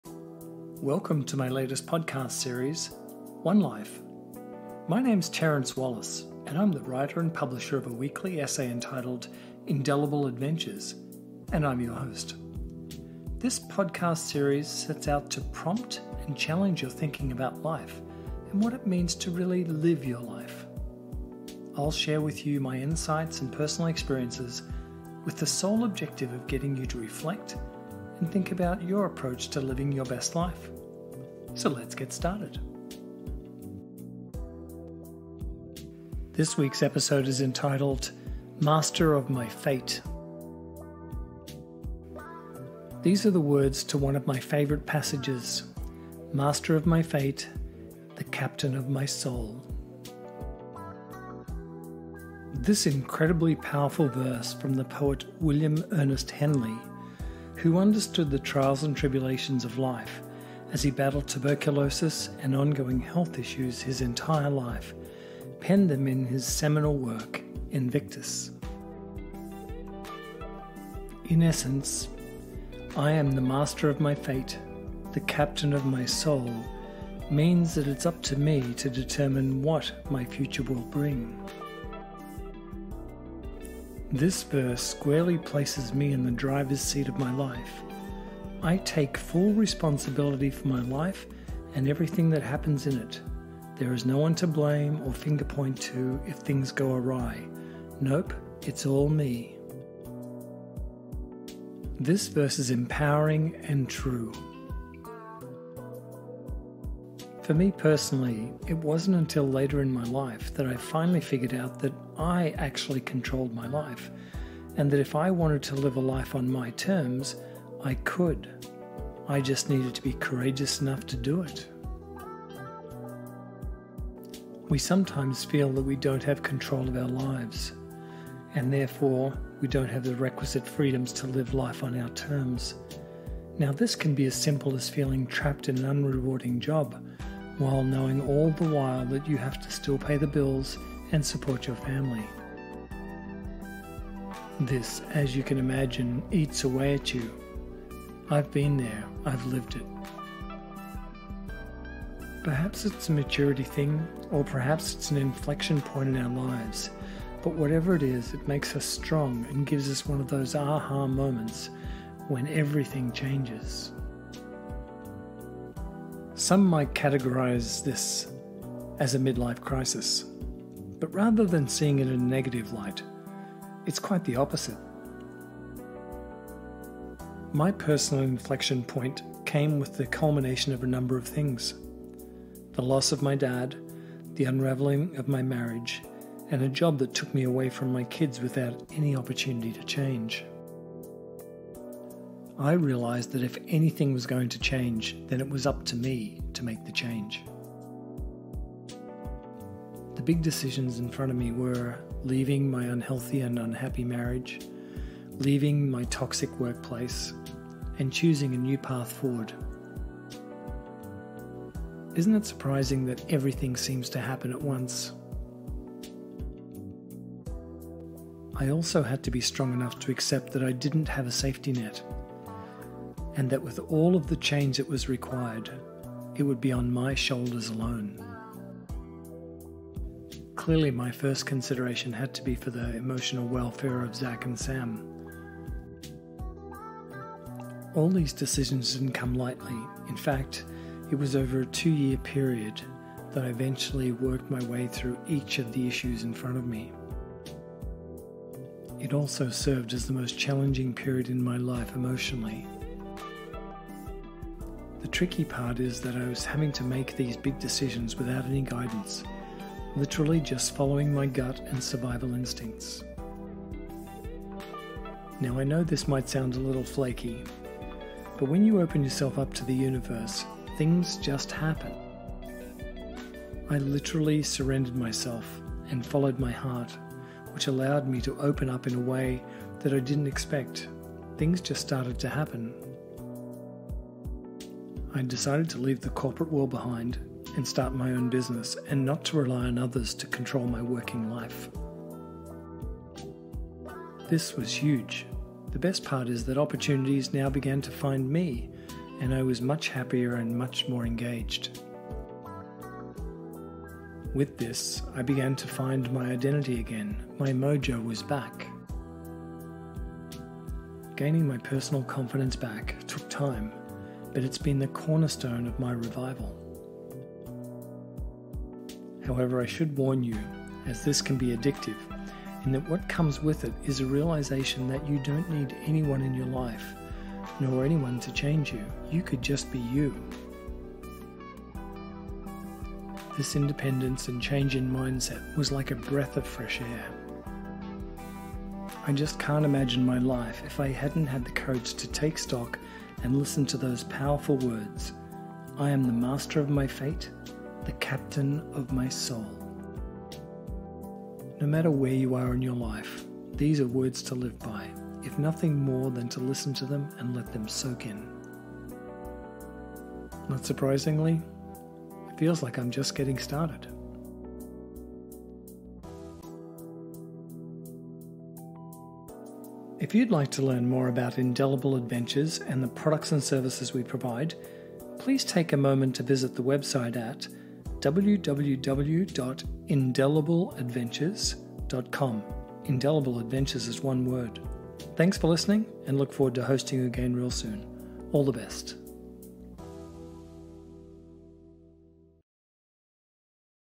Master of my fate...
"I am the master of my fate, the captain of my soul"
The immortal words from the poem Invictus by William Ernest Henley makes it clear to me that we are responsible for living our best life.  And that ultimately, our thoughts, words and actions dictate in what circumstance we choose to live it.
Come check out this week's podcast!